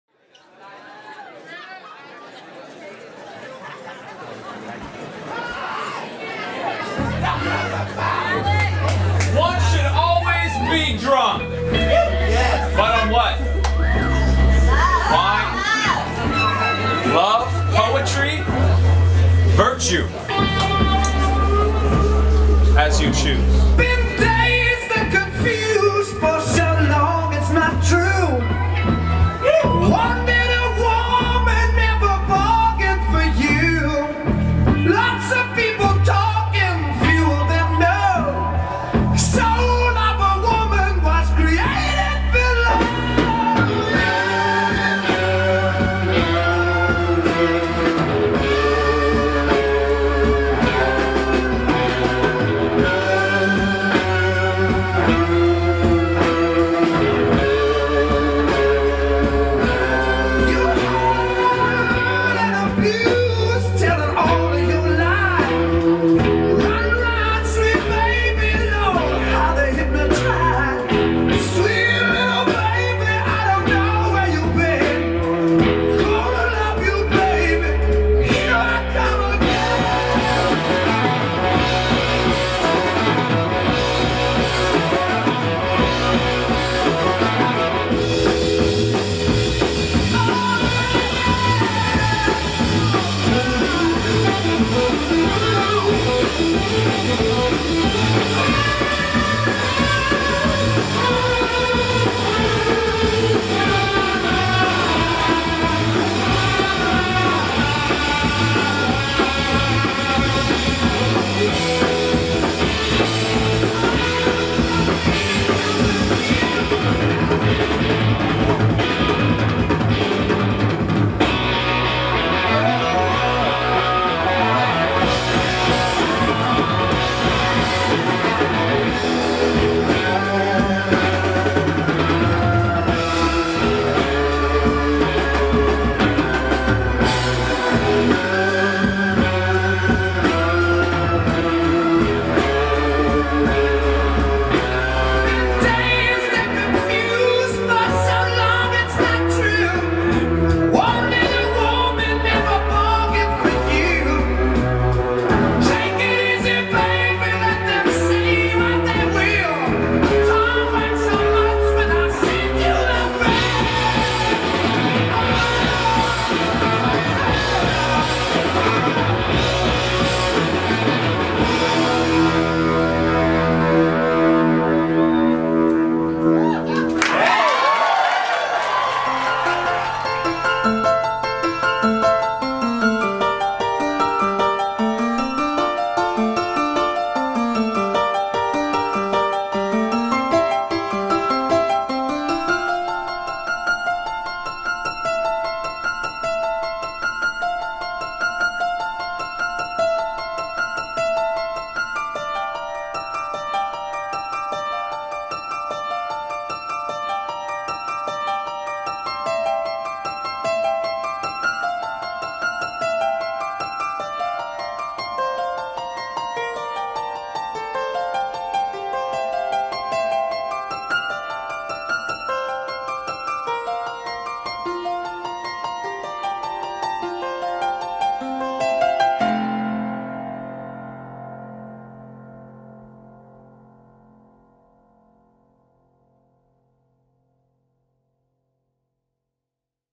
BC Beat | FALL 2014 – Laurie Kanyok
Choreographer | Laurie Kanyok
Performers | Laurie Kanyok, Alex Michael Stoll

BC Beat TEAM
creative director JENNIFER JANCUSKA

stage manager EMILY HAYES
dj JUSTIN JOHNSTON
lights STEVE GONZALEZ
videography JOSE FLORES
video editing THE BRINGABOUT PRODUCTIONS

AT HOME IN BROOKLYN